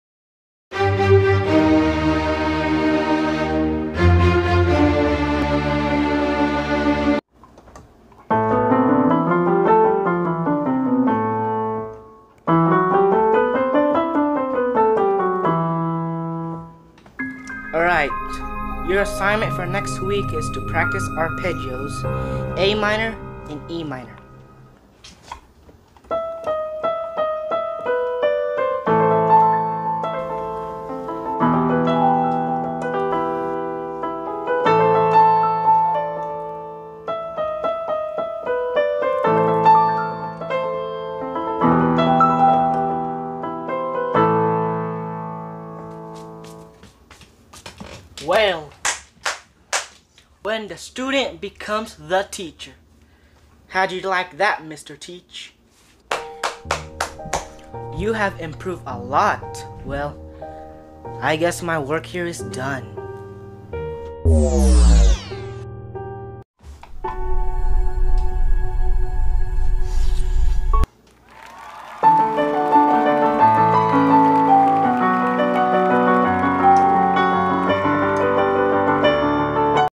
When your teacher teaches Arpeggios
A little skit of a teacher teaching Arpeggios.